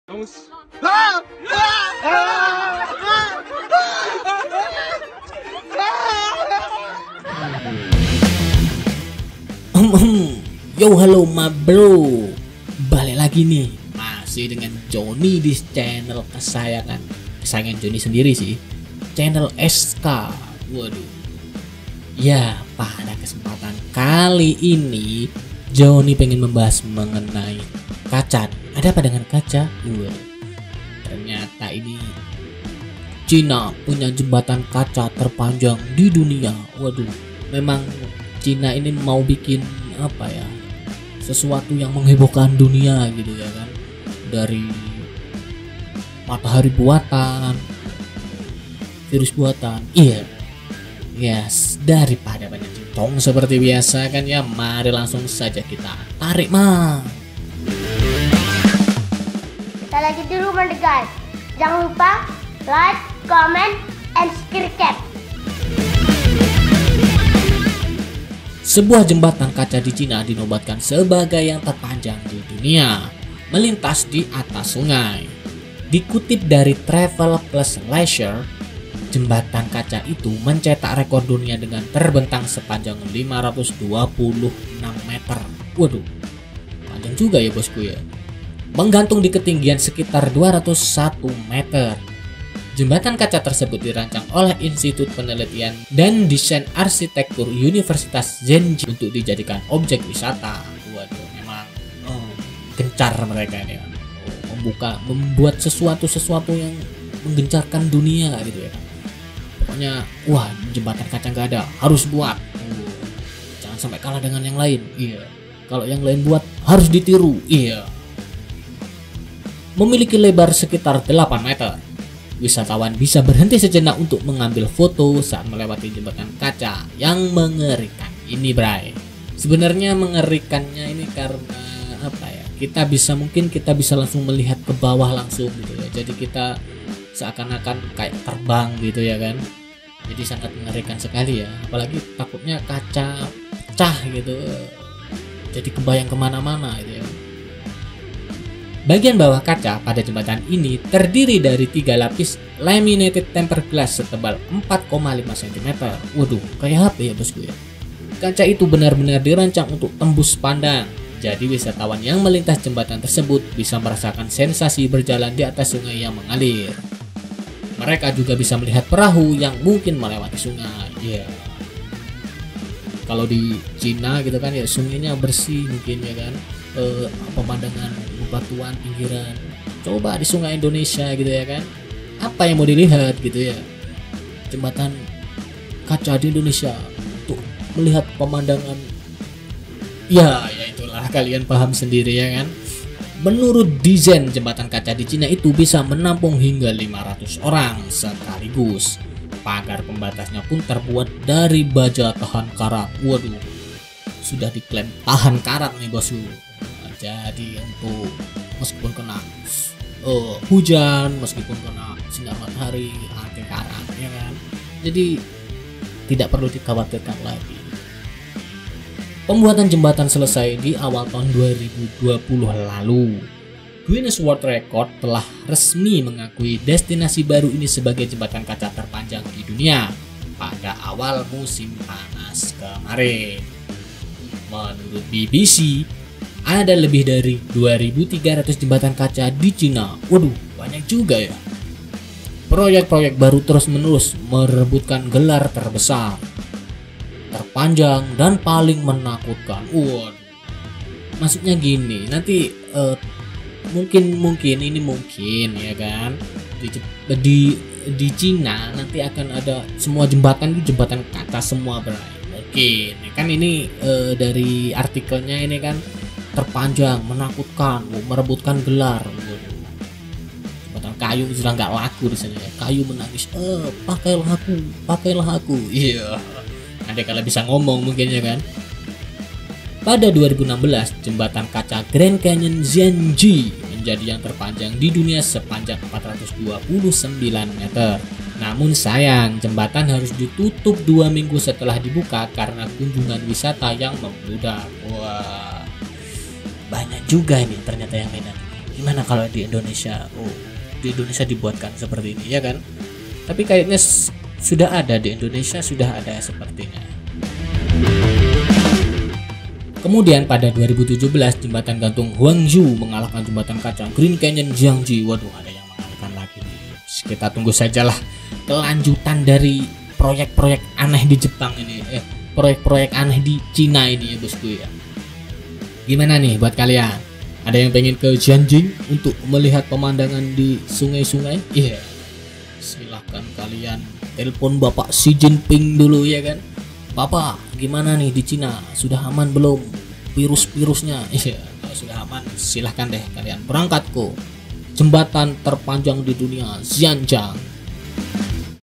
China Berlomba Membuat Jembatan Kaca Terpanjang di Dunia
China Berlomba Membuat Jembatan Kaca Terpanjang di Dunia, Sebuah jembatan kaca di China dinobatkan sebagai yang terpanjang di dunia. Melintas di atas sungai, jembatan kaca itu mencetak rekor dunia dengan terbentang sepanjang 526 meter, menggantung di ketinggian sekitar 201 meter, di atas Sungai Lianjiang.
Jembatan kaca tersebut dirancang oleh Institut Penelitian dan Desain Arsitektur Universitas Zhengjiang untuk dijadikan objek wisata. Memiliki lebar sekitar 8 meter.